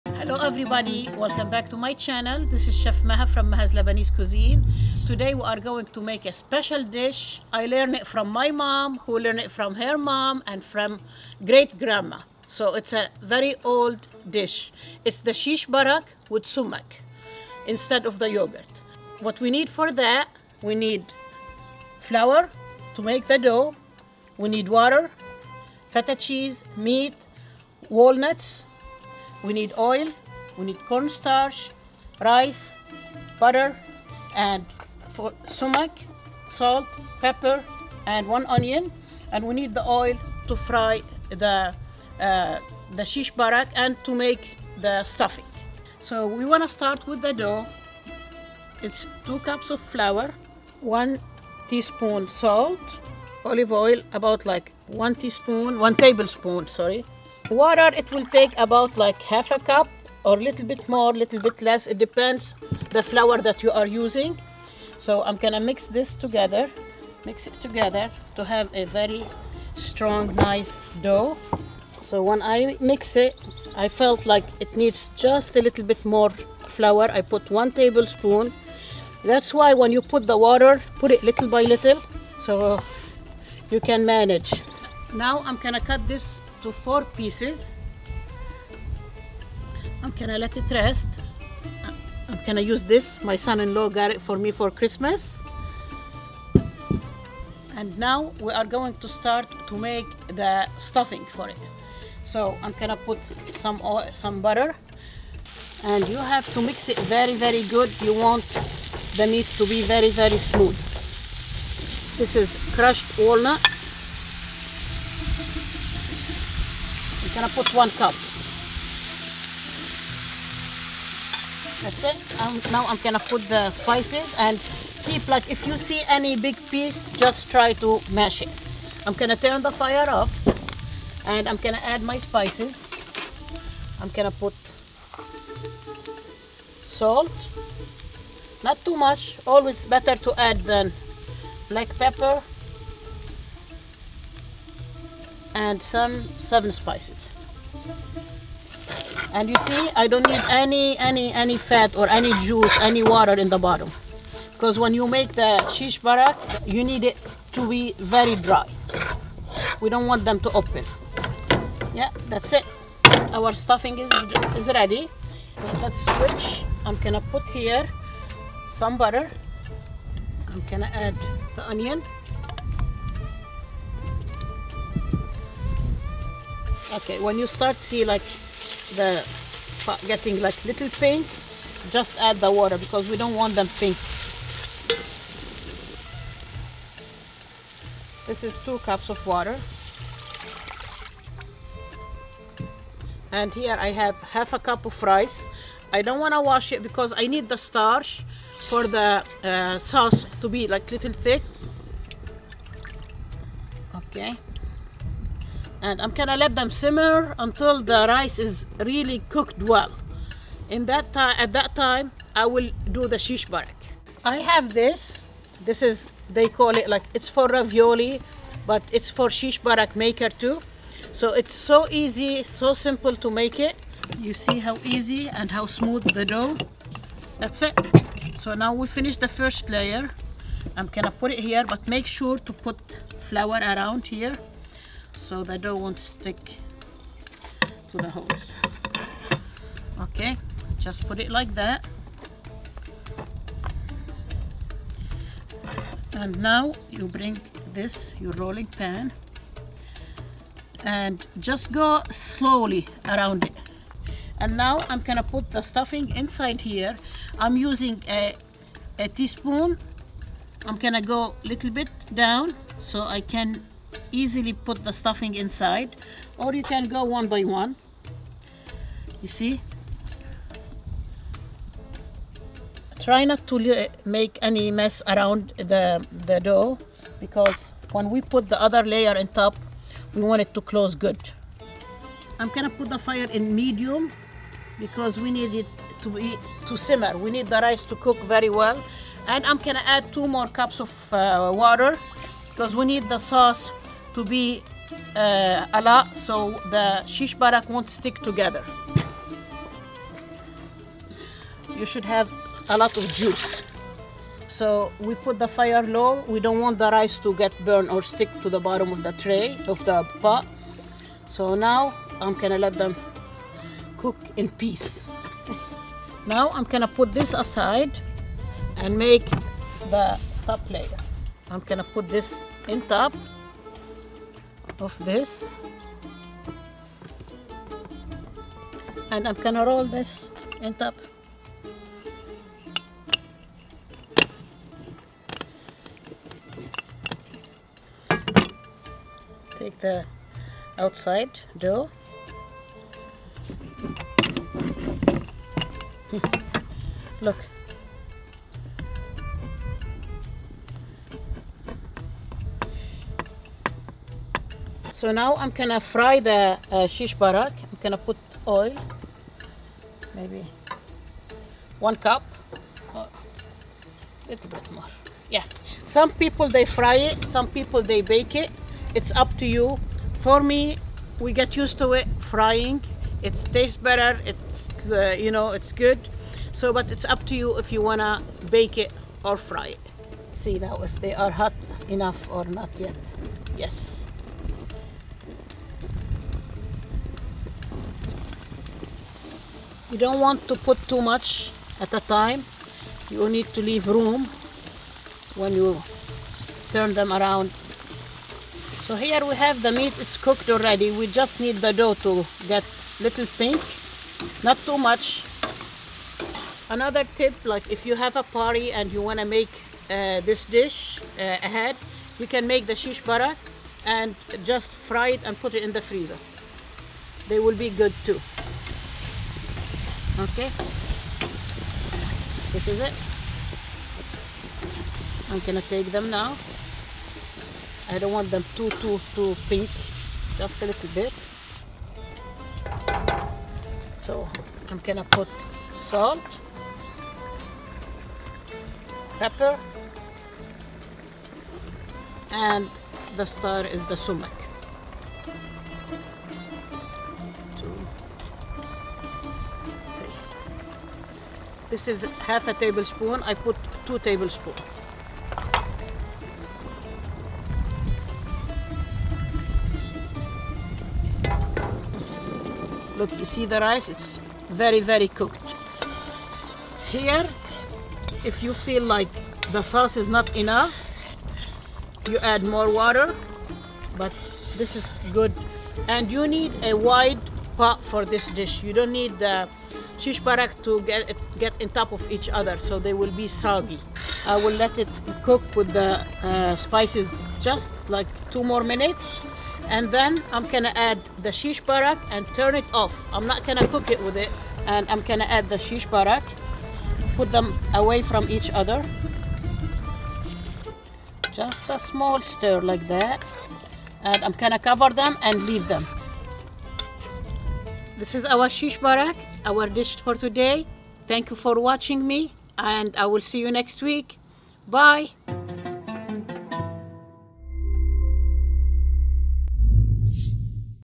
How to make Lebanese shish Barak with Sumac!
Shish barak is one of the best comfort food dishes in Lebanese Cuisine. It's super delicious, but does take a bit more effort. In this episode I'm showing you how to make the traditional beef stuffed shish Barak with sumac I hope you enjoy!!

Onion chopper

Outdoor tabletop grill (similar)

Traditional Lebanese bowls

Wooden large cutting board

Dutch oven